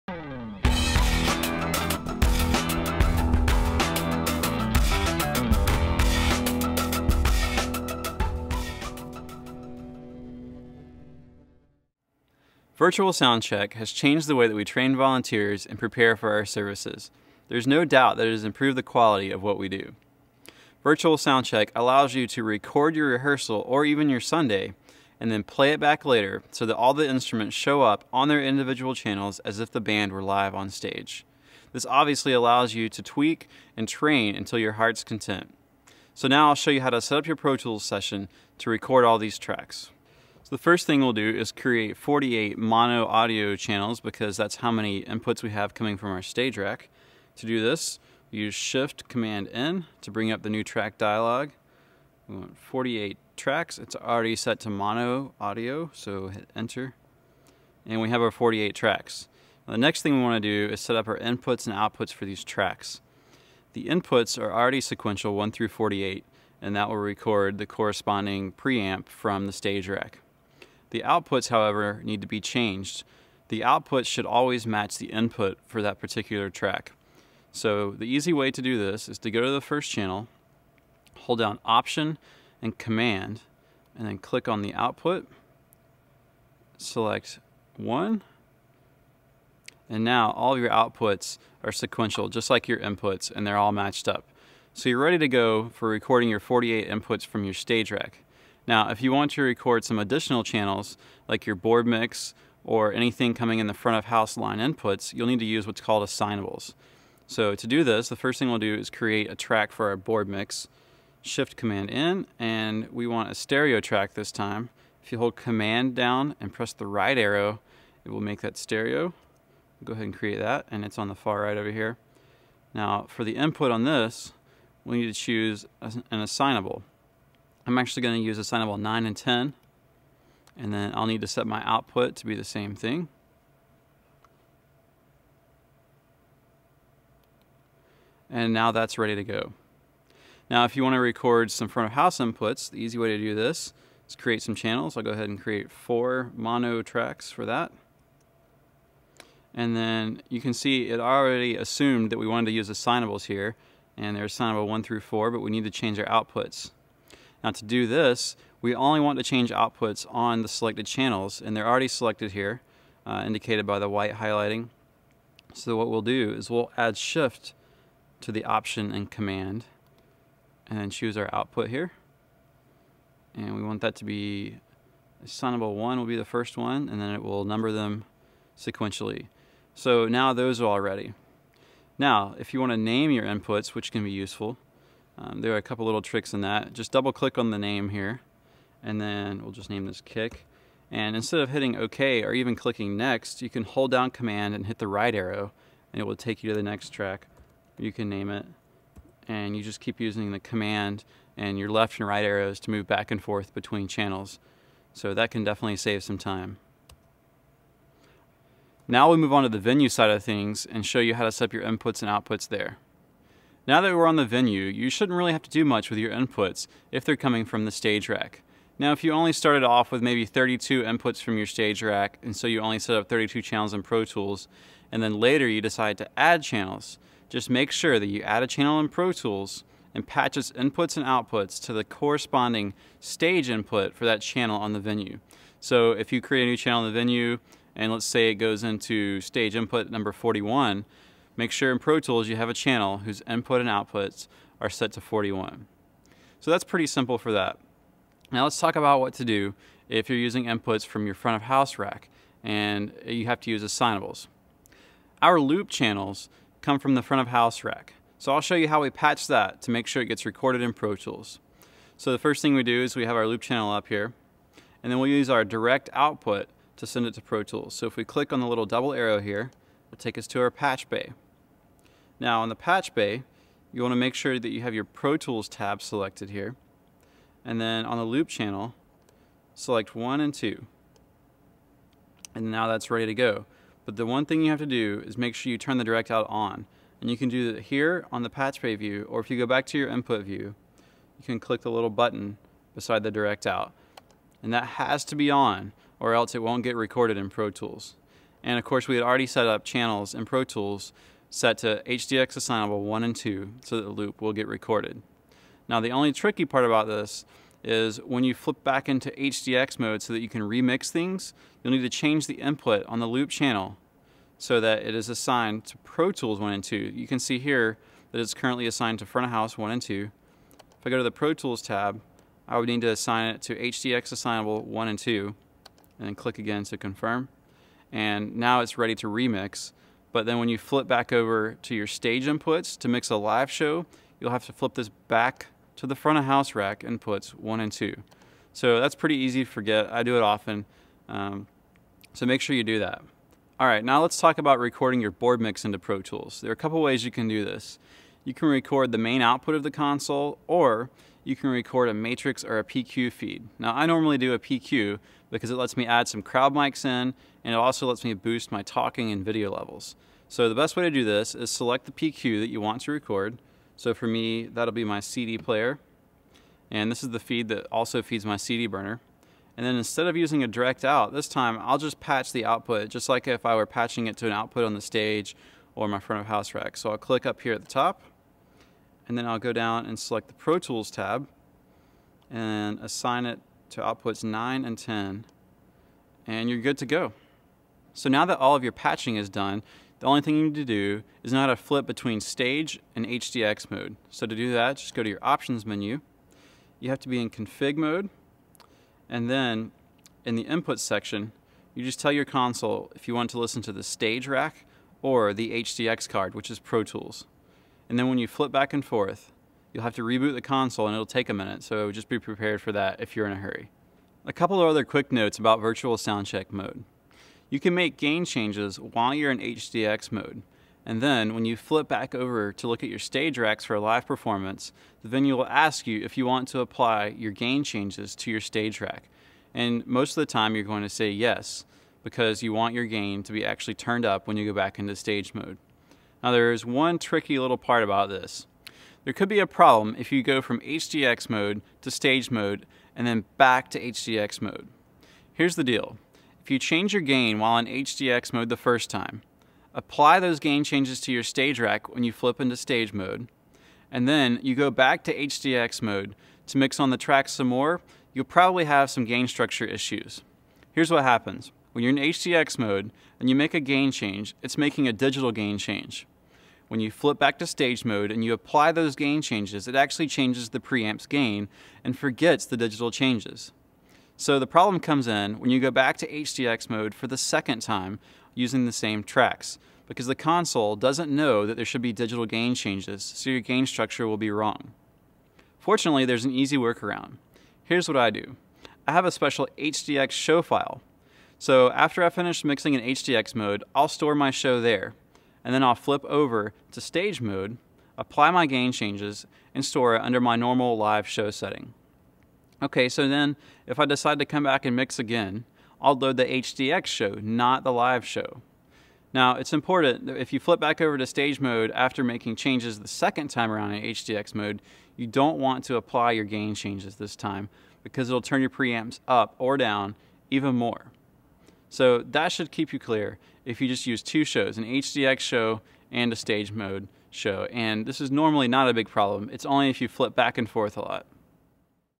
How to Use Avid's Virtual Sound Check
Virtual Soundcheck has changed the game for mixing live. In this video Dustin shows you how to setup a Digidesign Venue and Pro Tools to record and playback for a virtual soundcheck.